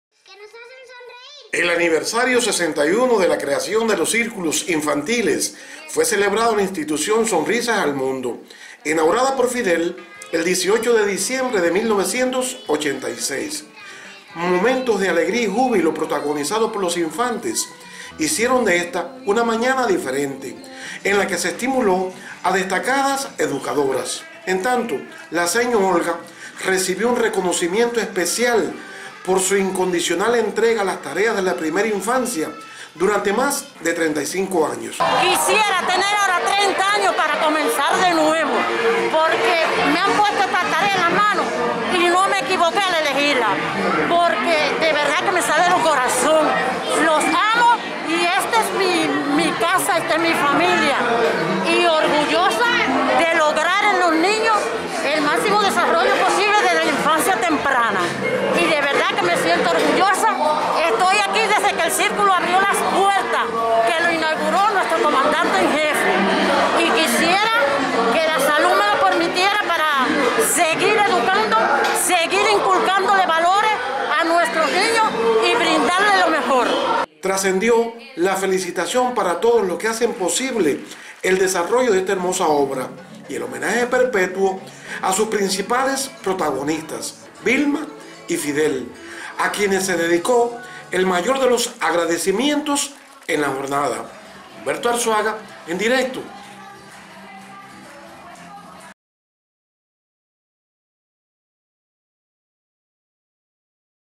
Celebran en Media Luna aniversario 61 de la creación de los Círculos Infantiles
Este 10 de abril se celebró el aniversario 61 de la creación de los círculos infantiles, un proyecto que la heroína Vilma espín materializó bajo el ideario del líder de la revolución cubana Fidel Castro.  Con motivo de la fecha se desarrolló una actividad cultural en el círculo infantil “Sonrisas al Mundo”, centro del municipio de Media Luna  inaugurado por Fidel el 18 de diciembre de 1986, y que atesora numerosos reconocimientos que lo distinguen como una de las instituciones destacadas en esta enseñanza en Granma.